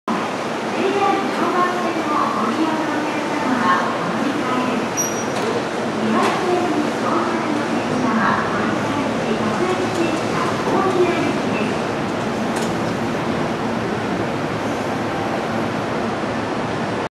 東武8000系トップナンバー8101F幕回し 柏→大宮
2013年6月15日撮影。東武8000系8101Fの柏駅到着後に変わる柏から大宮への正面幕回しです。東武野田線の大多数の幕編成は柏から大宮へ1コマで変わるのが多いのに対し、8101Fは柏幕と大宮幕の間に「大宮―柏」と「清水公園―柏」があります。